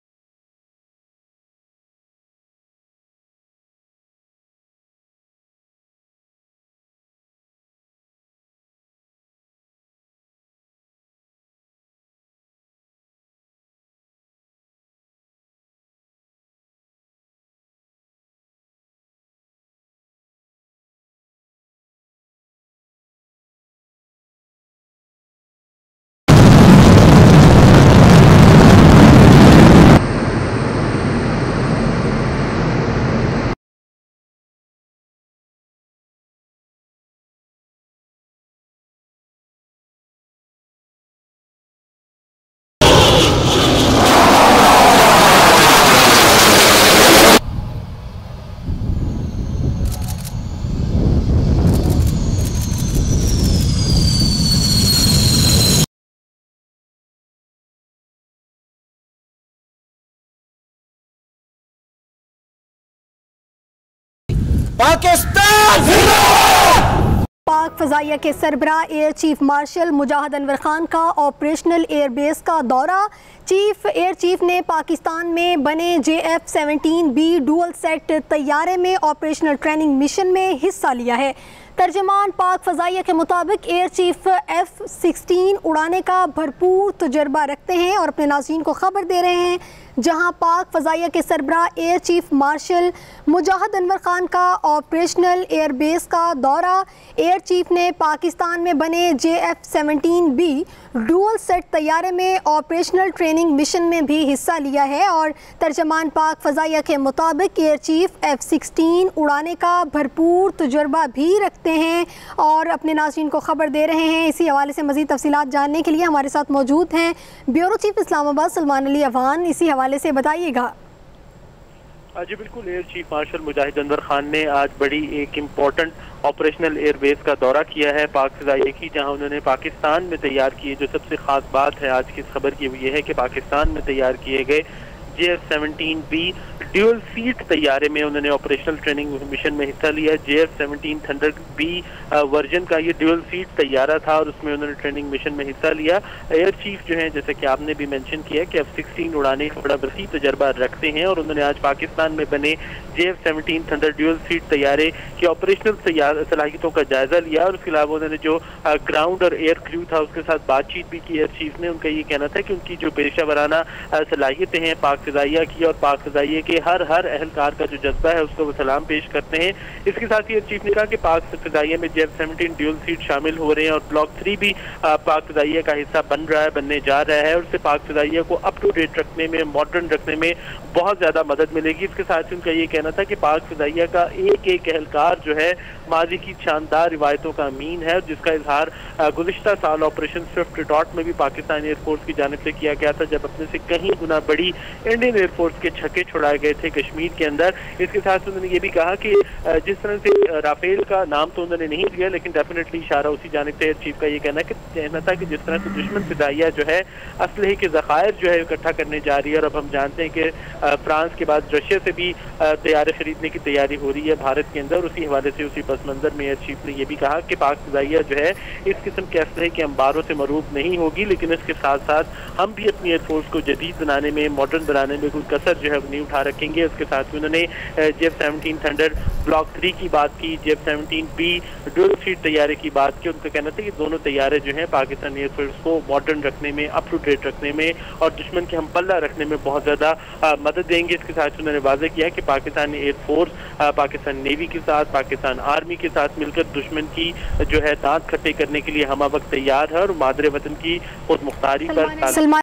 Air Chief Mujahid Anwer flies JF-17B | Visits operational air base
Air Chief Mujahid Anwer Khan flies JF-17B takes part in training  | Visits operational air base